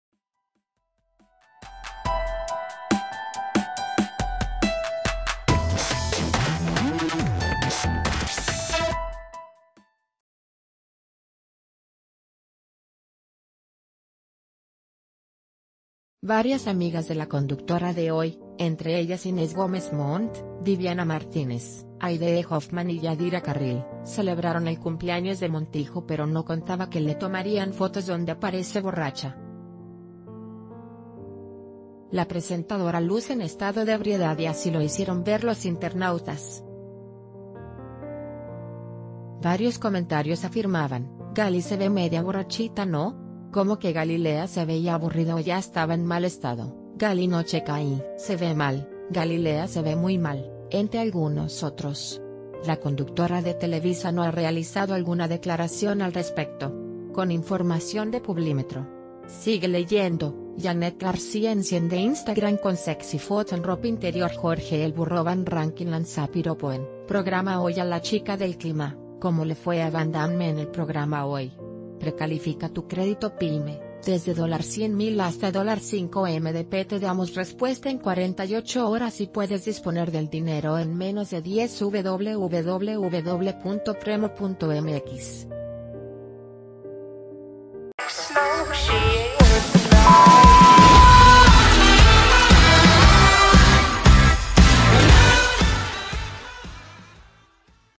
Filtran fotos de Galilea Montijo 'borracha' durante su cumpleaños
Varias amigas de la conductora de "Hoy", entre ellas Inés Gómez Mont, Viviana Martínez, Haydéé Hofmann y Yadhira Carrill, celebraron el cumpleaños de Montijo pero no contaba que le tomarían fotos donde aparece "borracha". La presentadora luce en estado de ebriedad y así lo hicieron ver los internaut...